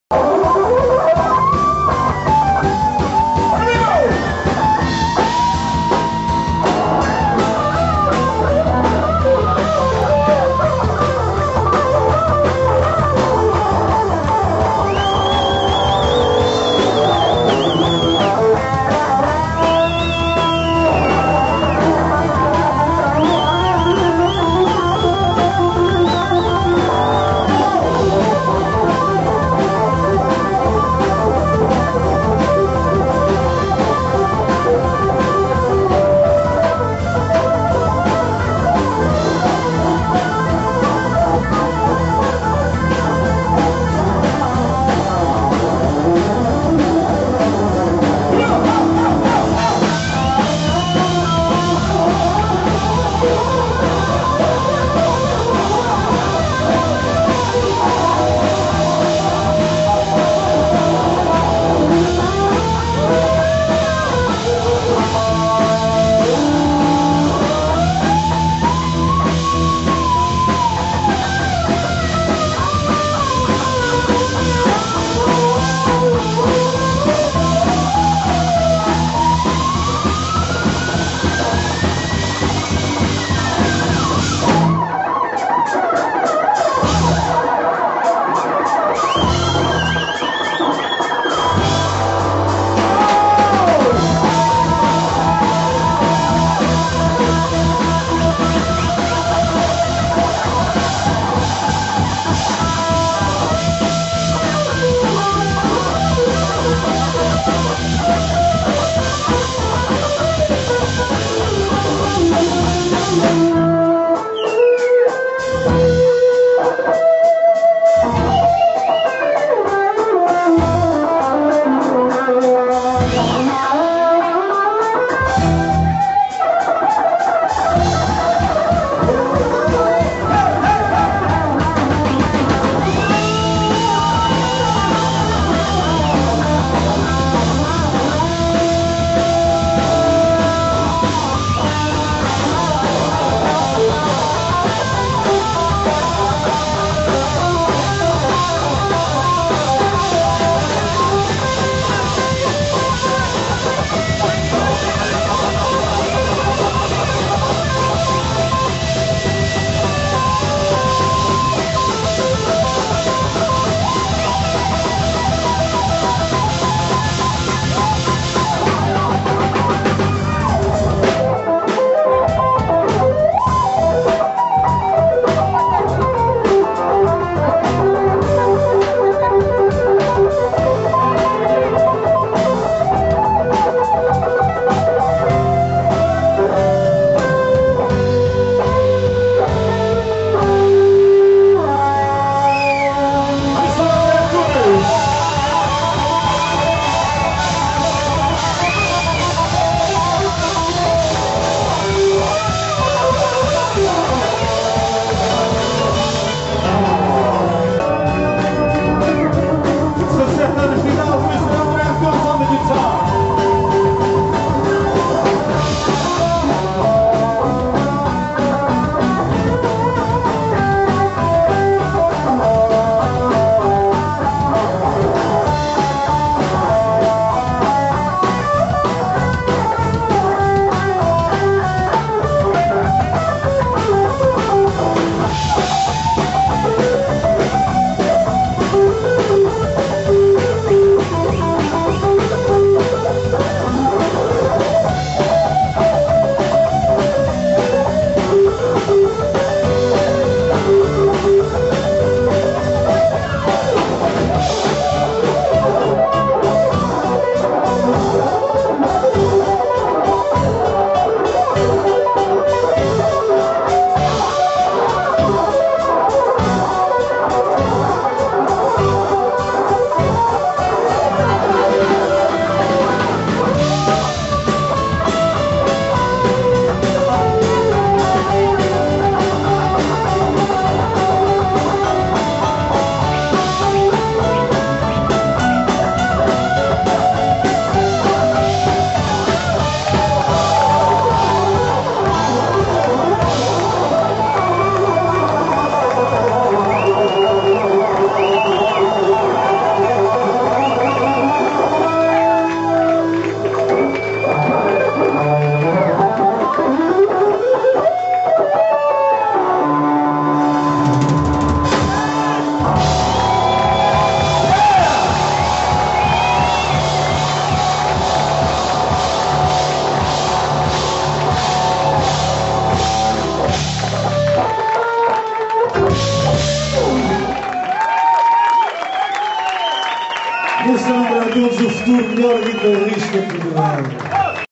André Antunes
André Antunes faz solo de guitarra para o convidado especial Nuno Guerreiro no Vitorino´s Bar em Santarém.